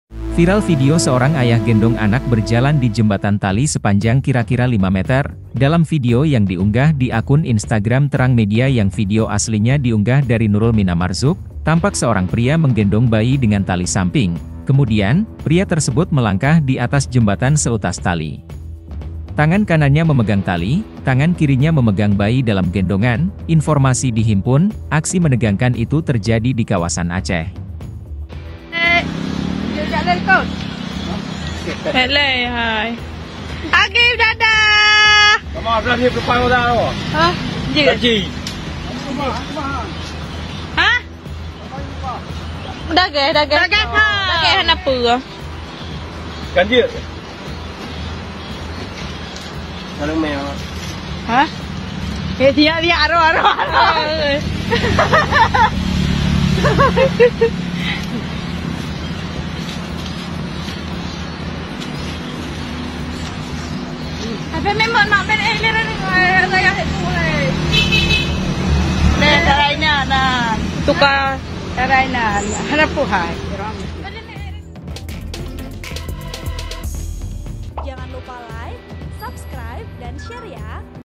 VIRAL Video Seorang Ayah Gendong Bayi Lewati Jembatan 'Sirotol Mustakim' di atas Sungai Deras
TRIBUNJABARVIDEO- Viral video seorang ayah gendong anak berjalan di jembatan tali sepanjang kira-kira 5 meter.

Dalam video yang diunggah di akun Instagram Terang Media yang video aslinya diunggah dari Nurul mina marzuk, tampak seorang pria menggendong bayi dengan tali samping.

Kemudian, pria tersebut melangkah di atas jembatan seutas tali. Tangan kanannya memegang tali, tangan kirinya memegang bayi dalam gendongan.

Informasi dihimpun, aksi menegangkan itu terjadi di kawasan Aceh.(*)